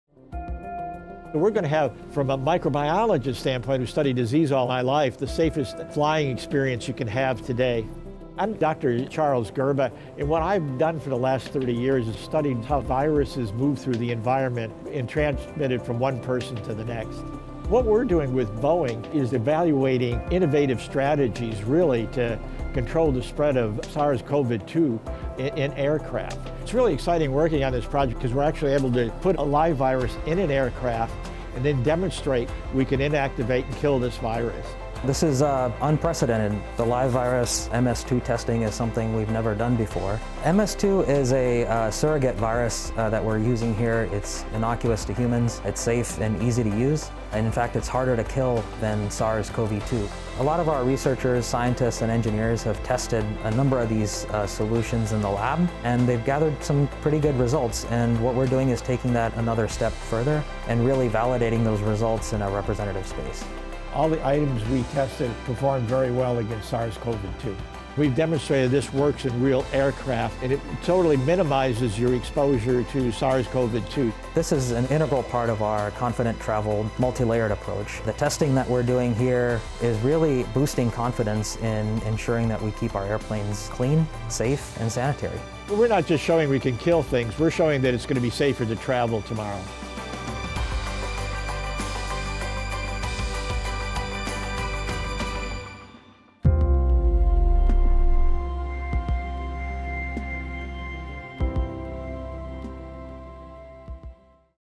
Disinfecting Products Validated with Innovative Live Virus Test
Boeing and the University of Arizona partnered to conduct a unique study. We put a live virus on an airplane to validate the effectiveness of our airplane cleaning recommendations. The virus, MS2, is safe to humans and harder to kill than SARS-CoV-2, the virus that causes COVID-19.  Learn more about the first-of-its-kind test, and the results.